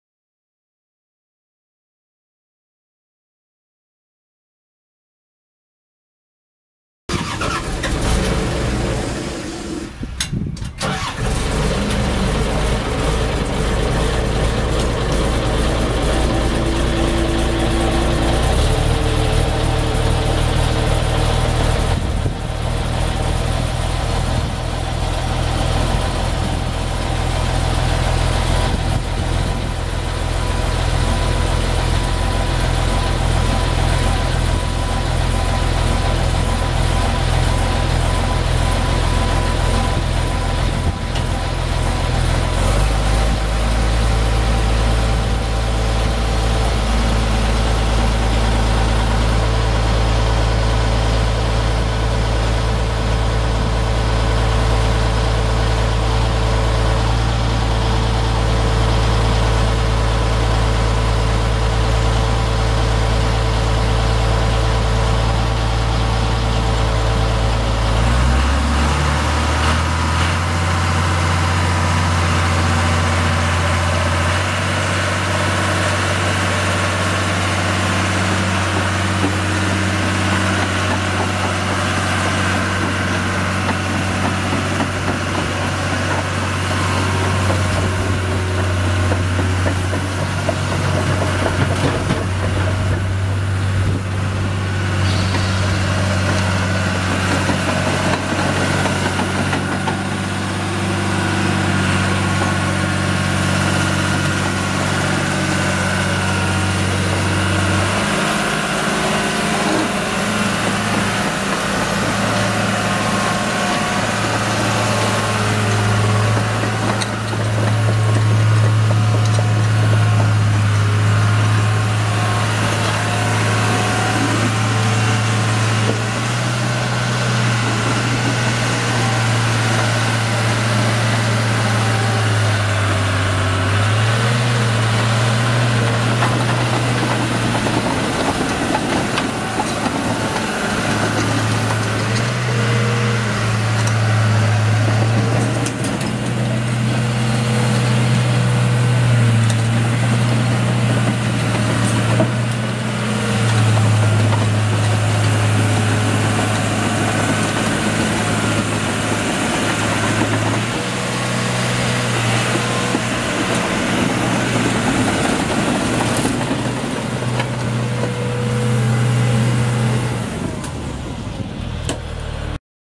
1994 Caterpillar 939 track loader for sale | sold at auction December 31, 2014
1994 Caterpillar 939 track loader
2,976 hours on meter
Caterpillar 3046 six cylinder diesel engine
Serial 5XK00596
Arrangement No. 8Y8160
3F-3R gears
Canopy
7&#039;W eight tooth bucket
18"W tracks
21,640 lbs. base operating weight
Serial 9GL00120

It was located in Ashland, MO, and we sold it to the highest bidder for $18,150.